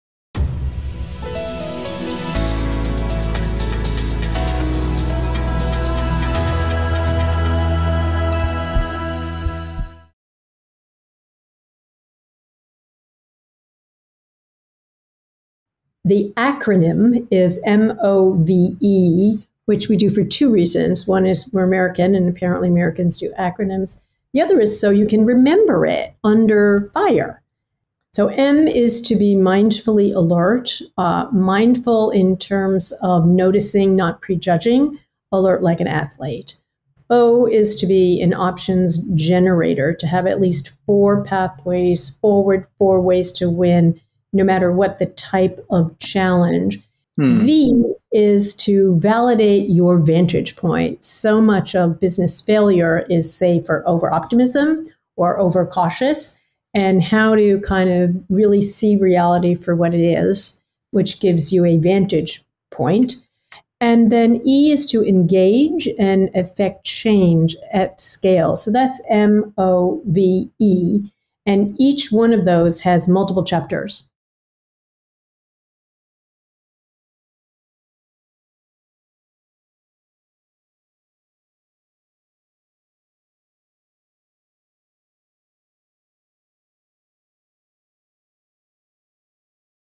P2P Extract #77 - Carol Kauffman & David A. Noble - The MOVE Framework
Viktor Frankl, Holocaust survivor and the author of Man's Search for Meaning says “Between the stimulus and response, there is a space. In that space is our power to choose our response. In our response lies our growth and our freedom". Carol and David pick it up from there and tell us how we can make the space count using their MOVE framework.

Sign up for the email newsletter (and see the past 20)